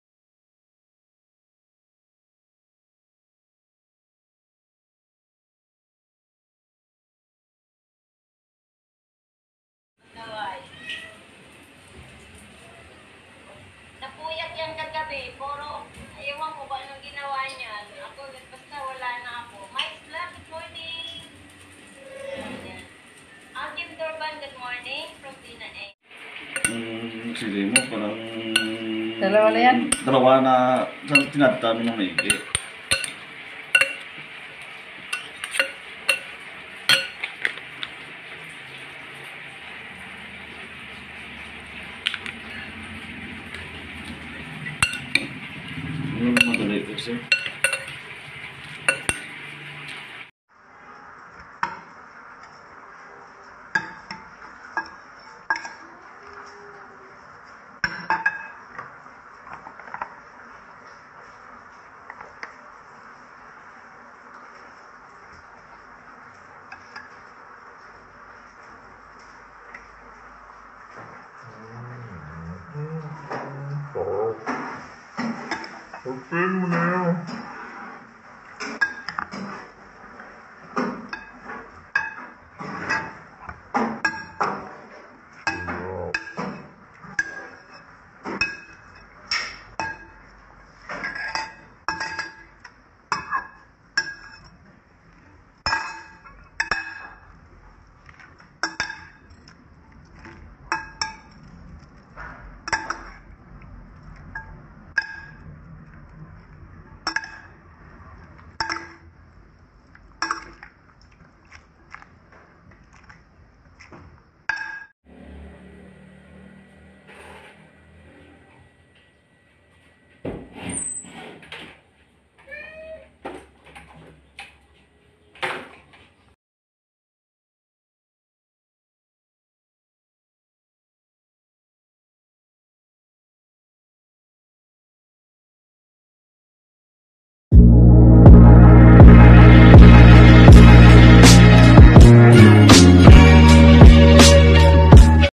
Ensaladang Lato o Sea Grapes | Jhayunah
Ensaladang Lato or Seaweed Salad is a type of Filipino salad prepared with Sea Grapes or Caulerpa, tomatoes, onions and fermented shrimps, a popular salad that is usually served with grilled seafood